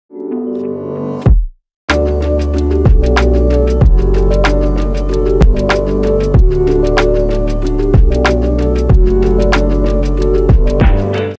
iPhone 14 Pro model with rotation in Twinmotion.
Object animation with rotation in Twinmotion.